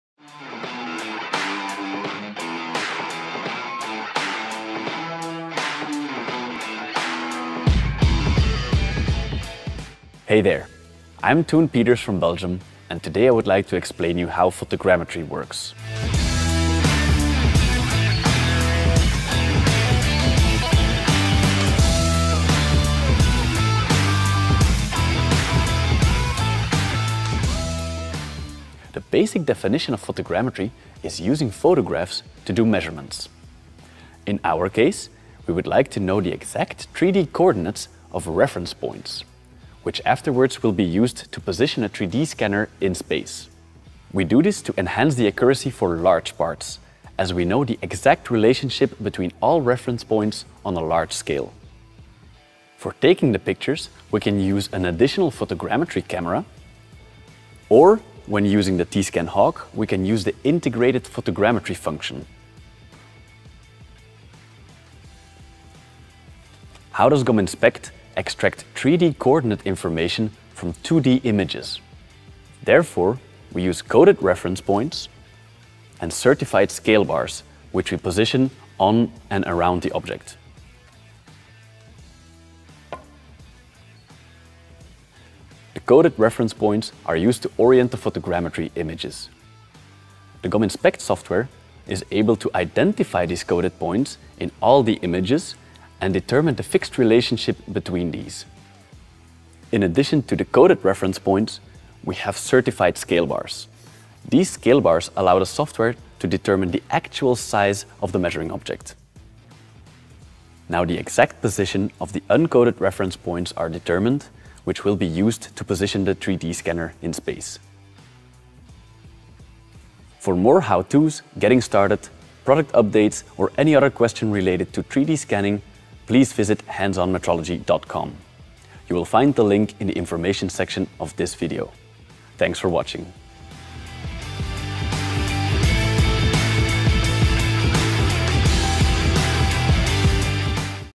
Jak działa fotogrametria i dlaczego jej potrzebujesz
Fotogrametria to techniczno-technologiczne rozwiązanie, umożliwiające dokładne odtworzenie rzeczywistości w przestrzeni wirtualnej. W przypadku skanowania 3D służy ona do uzyskania precyzyjnych współrzędnych punktów referencyjnych, które w późniejszych etapach tego procesu, wykorzystywane są do umiejscowienia skanera 3D w przestrzeni. Fotogrametria umożliwia zwiększenie dokładności pomiaru dużych części, poprzez dostarczanie szczegółowych informacji na temat położenia punktów referencyjnych w przestrzeni pomiędzy poszczególnymi punktami referencyjnymi.